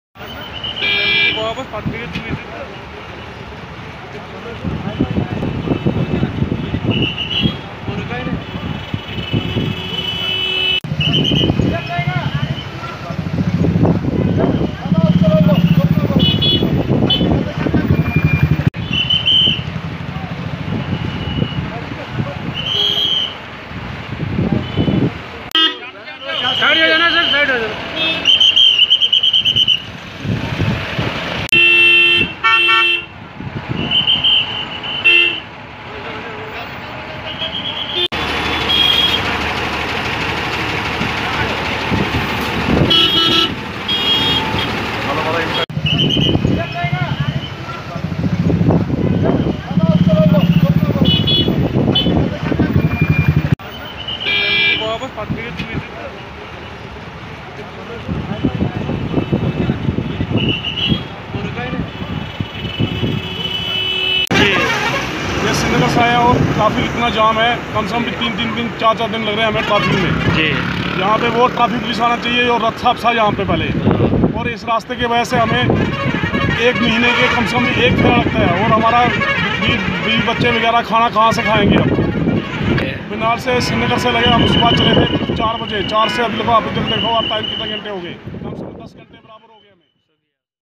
Jammu Srinagar National Highway par traffic jam jare Musafir pareshan Jammu Srinagar National Highw
Traffic Jam